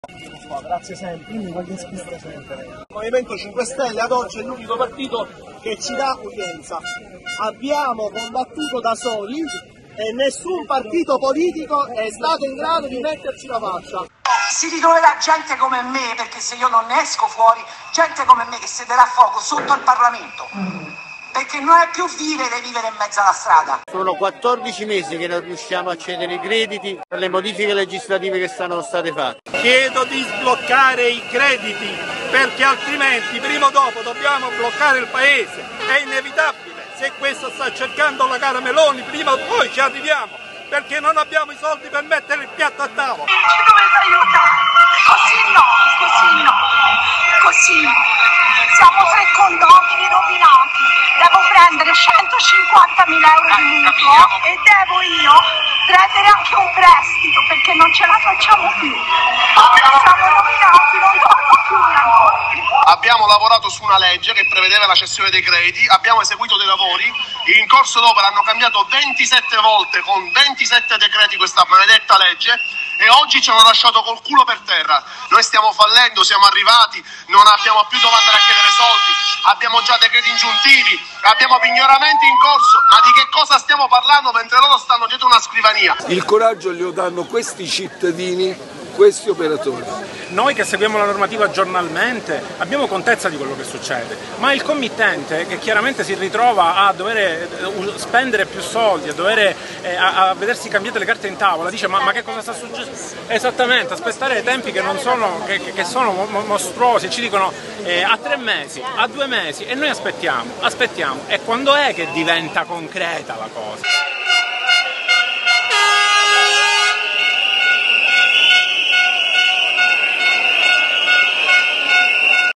Gli esodati del Superbonus
Un corteo di protesta ignorato dai tanti, una proposta in 10 punti ferma sul tavolo del Mef. Italiani colpiti duramente dai crediti fiscali incagliati. Lavori bloccati e mai finiti